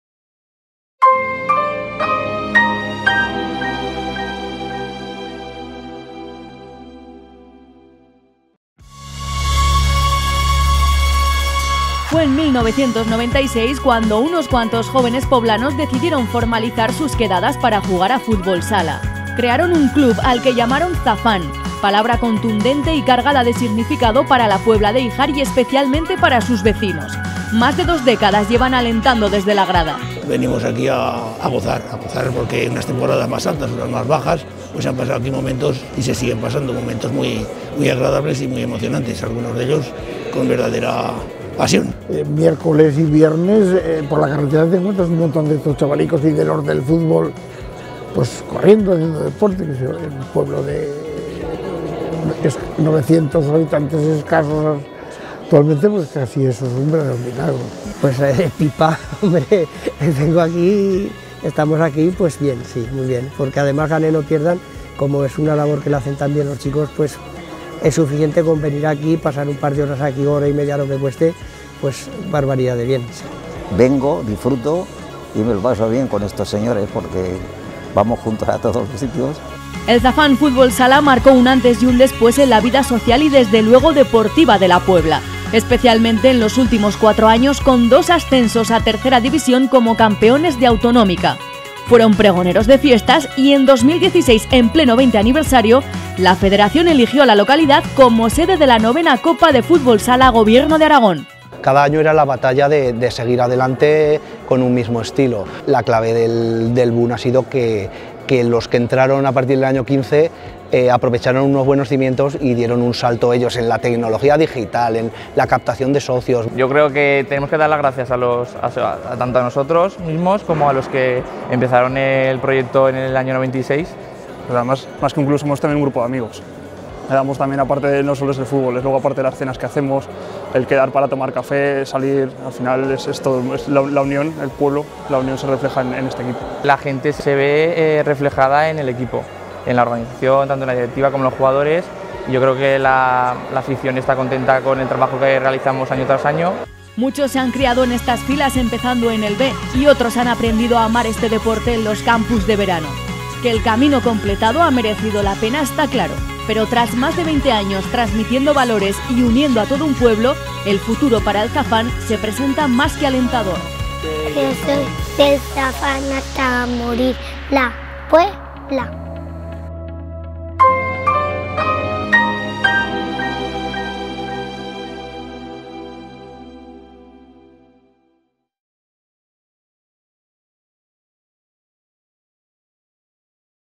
ZAFÁN FÚTBOL SALA - Nominación a Bajoaragonés del año 2018
Los clubes deportivos mantienen la cantera deportiva inculcando valores sanos entre los jóvenes y ayudándolos a superarse. Aquí puedes ver un vídeo presentación de uno de los nominados en la sección de Deportes de los premios Bajoaragonés del año 2018.